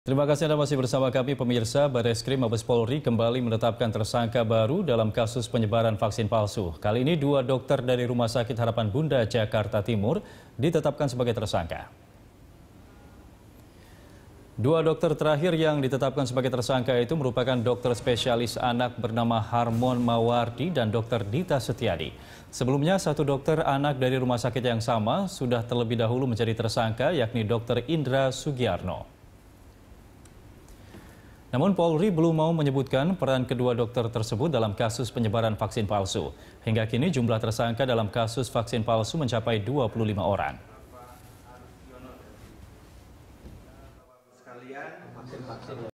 2 Dokter RS Harapan Bunda Jadi Tersangka Baru Kasus Vaksin Palsu
Bareskrim Mabes Polri kembali menetapkan tersangka baru dalam kasus penyebaran vaksin palsu. Kali ini dua dokter dari RS Harapan Bunda ditetapkan sebagai tersangka.

Facebook.com/BeritaSatuTV
Youtube.com/BeritaSatu